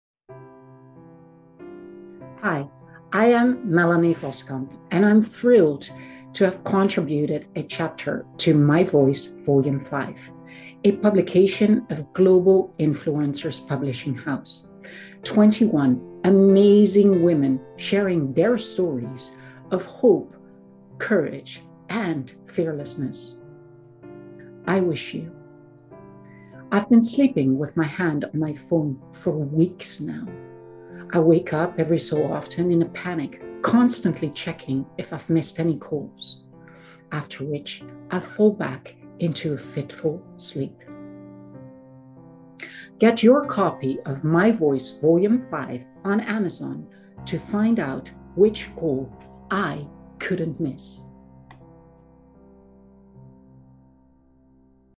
Presenting Melanie Voskamp - Co-author of MyVoice Vol.5
MyVoice Vol. 5 - Inspiring Stories of Hope, Courage & Fearlessness is an ensemble volume of moving stories of life-changing decisions that can change every aspect of our surroundings, family dynamics and future happiness.

The twenty one women who have written chapters in this book have determined not to be beaten by obstacles placed in their path but instead faced their adversity by making life for themselves and those around them better – hope, courage and fearlessness are their underpinning strengths.